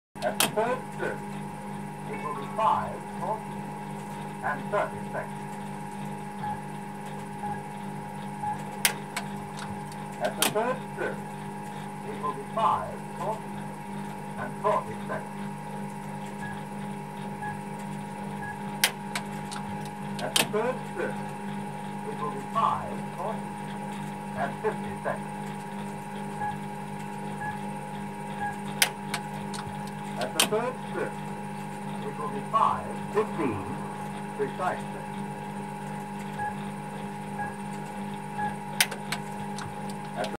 Mechanical talking clock from the 1950's PMG
This is the 1950's mechanical talking clock from the Post Master General as it was then before Telecom and later Telstra were formed. This the voice that we all used to listen to on 1194. This unit uses a 50 Hz synchronous drive to keep everything at the right time and reads the sound off glass discs. The mechanical system chooses which tracks to play. Remember this is 40 years before the CD optical discs. This machine is absolutely fascinating to watch and listen too with amazing monkey motion.